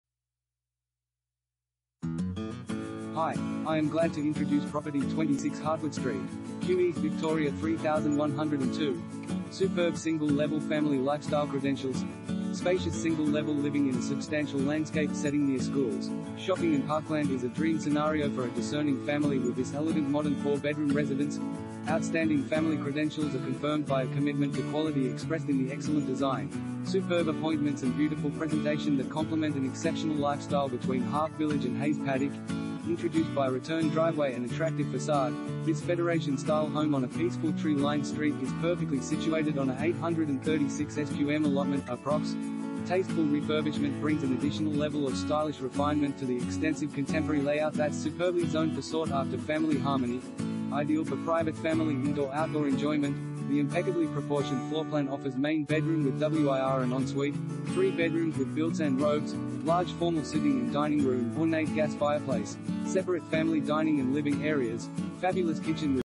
26 Hartwood Street, Kew East VIC 3102 | AuPropertyReport.Com
26 Hartwood Street, Kew East VIC 3102
Property Type: house
Bedrooms: 4
Bathrooms: 4
Car Space: 2
Superb Single-Level Family Lifestyle Credentials